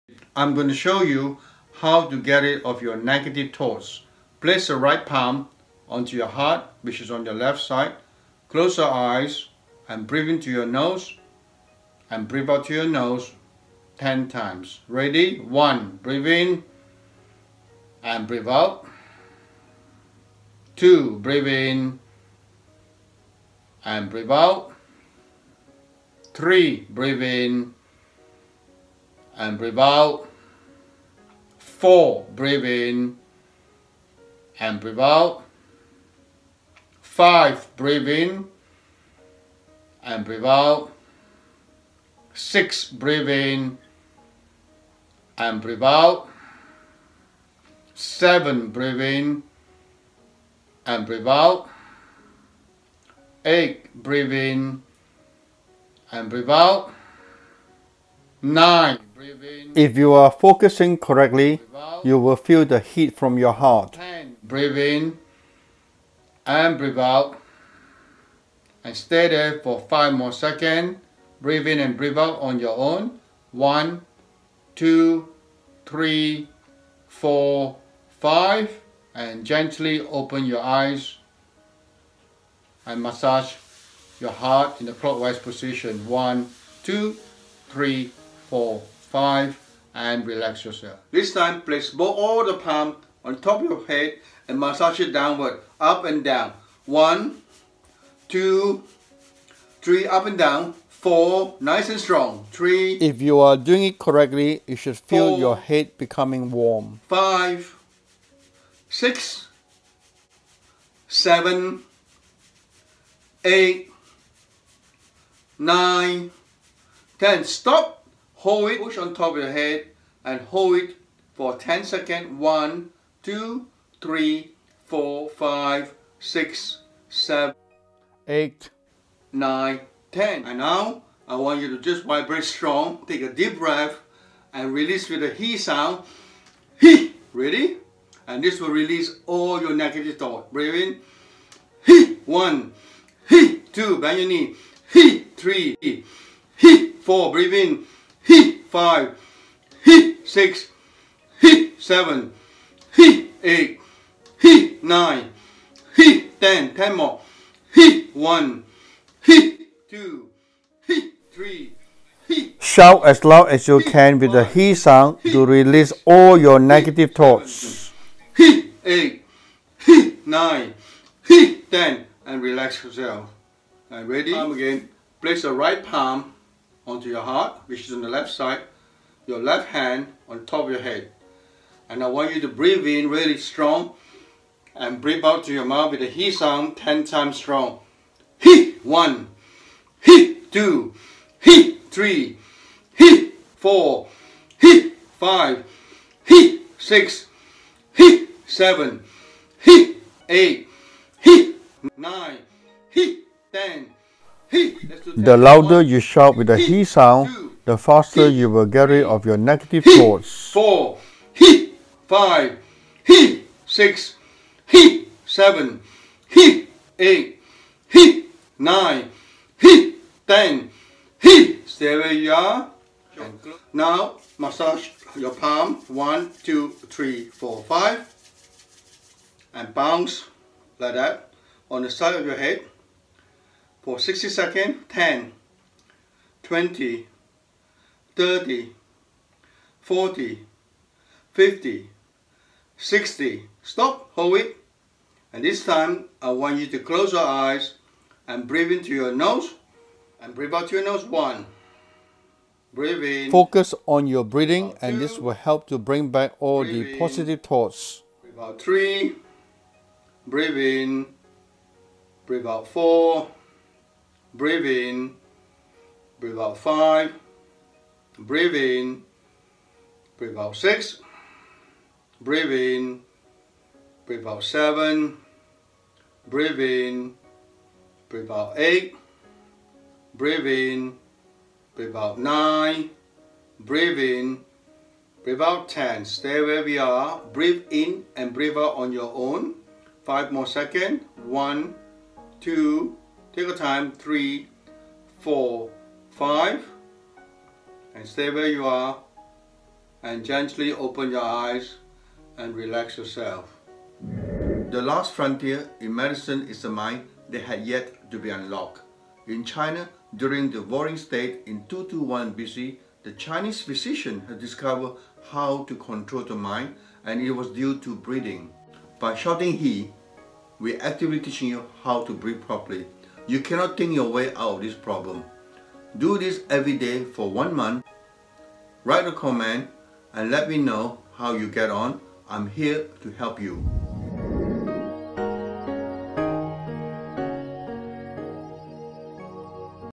How to get rid of your negative thoughts in 5 simple steps
🎁 [FREE CASE STUDY] - How I Get Rid of Chronic Congestion and Breathe Better:

Negative thinking is a thought process where people tend to find the worst in everything. You often feel.
Guilty, thoughts of struggle, worrying, depression, loneliness, harming others, black spirit, voodoo, sadness, over thinking, anger and suicidal.

The available, medication is OK but there can be side effects. The medicine just surprising the problems and if you stop taking the medicine, it will come back again.

Your tube videos that are available with theory like thinking positive, programming are only a short term solution and will not be effective in the long term. You are not solving the root cause of the problem.
You need to get rid of your negative thoughts and feelings out of your mind and body.

Negative thoughts is a defence mechanism that help us to protect our self.
In case we get hurt or harmed, physically and emotionally.
But this can be a bad habit because our thoughts can become reality.

Most people who experience negative thoughts, can seriously affect the way they think about them-self. All these negative thoughts can affect their work, study and relationship.

Here are some signs of negative thoughts.

1. Complaining in all situation.
2. Being angry or on-edge most of the time.
3. Feeling of depression and anxiety
4. Always talk down of others.
5. Difficult to communicate with people
6. Interactions with people becoming difficult or exhausting
7. Becoming more critical of self and others

How do you stop thinking negative thoughts?
1. Keep Good Company.
2. Exercises
3. Go out doors to do things, visiting, shopping, to the beach etc.
4. Do your breathing exercises daily.
5. Just take a few steps back and relax.

In the beginning you might some difficult following the program, you must persist and carry on doing it for at least a month. After four weeks, you should start to feel better. The reason it takes a month because you need time to get rid of negative thoughts gradually.

This video will help you to understand how to get rid of your negative thoughts gradually in the natural method.